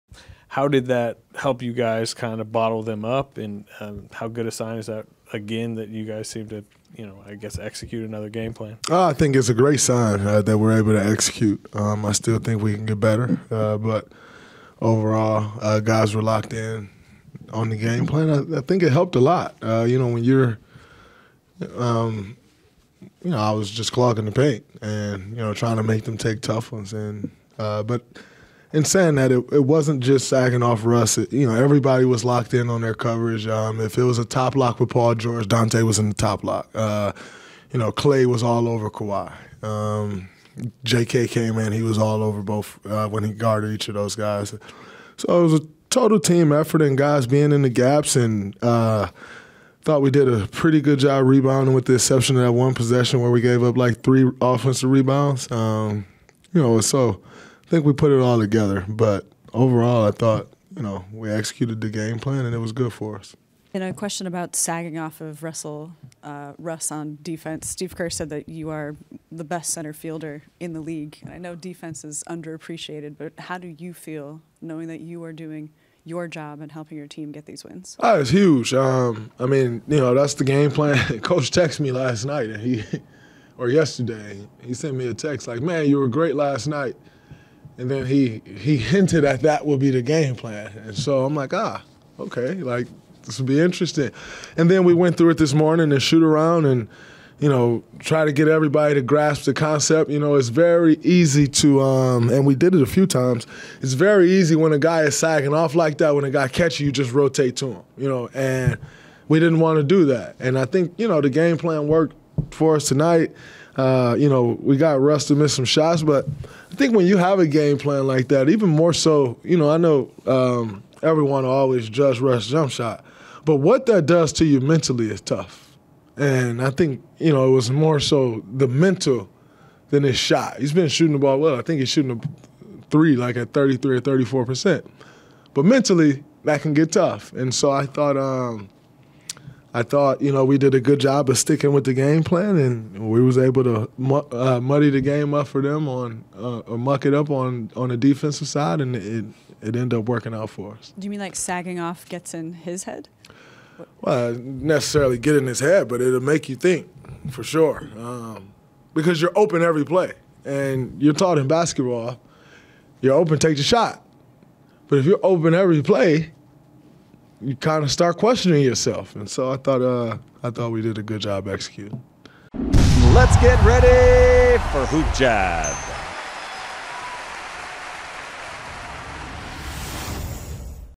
Draymond Green On Getting In Russell Westbrook's Head While Guarding Him. HoopJab NBA
Former All Star Forward Draymond Green speaks to the media following the Golden State Warriors 115-91 win over the Los Angeles Clippers. Green finished the game with 11 points, 9 rebounds, & 9 assists.